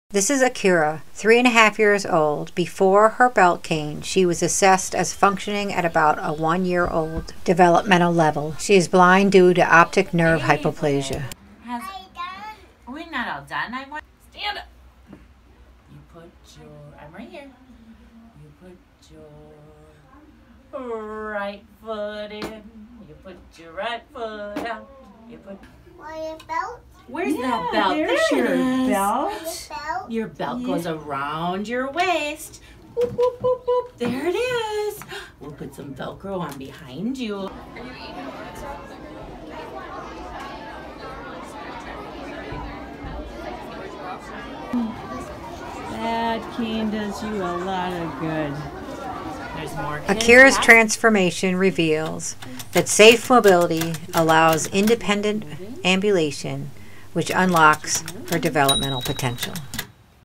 Safe mobility unlocked Akira’s developmental potential
Akira is blind due to optic nerve hypoplasia. Before belt cane she was stroller dependent, uninterested in her peers and school activities. Her teacher reported she took to the belt cane like a duck to water. She became a confident walker and an interested student. To see more of Akira’s story